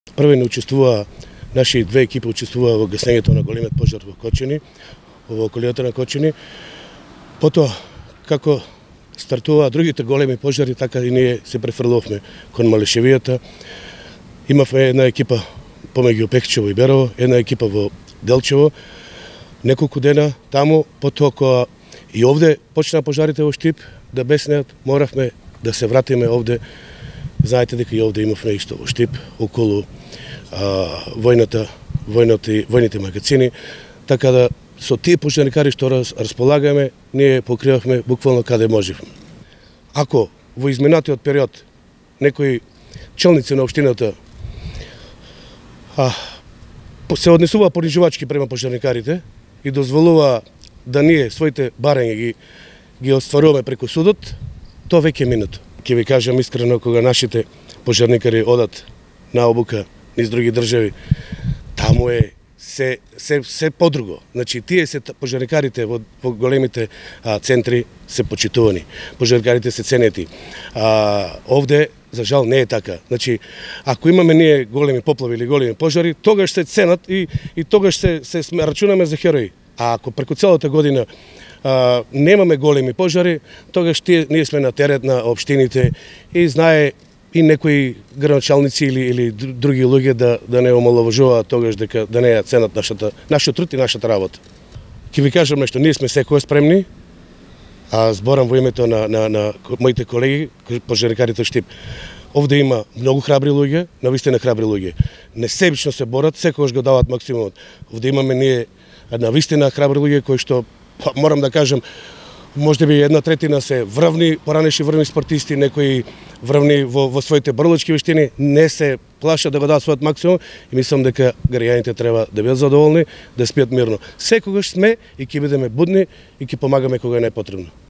Тони Ѓоргиев, пожарникар: Граѓаните можат да спијат мирно
Тони Ѓоргиев е пожарникар од Штип. Го прекина својот одмор по деноноќна битка со пламените јазици низ источните краишта на нашата земја, за да се сретне со нашата екипа.
Ова се само неколку акценти од разговорот во кој откриваме дека неговите колеги пожарникари се соочуваат со ризиците од пожарите храбро, без оглед на ограничените ресурси. Тој со нас сподели дека штипските пожарникари се храбри луѓе, меѓу кои има и поранешни врвни спортисти.
Граѓаните можат да спијат мирно, уверува Тони. Ние му веруваме.
Интервјуто го водеше Војо Маневски (ТВ Ирис), а снимаше Атанас Петровски (ЦИВИЛ)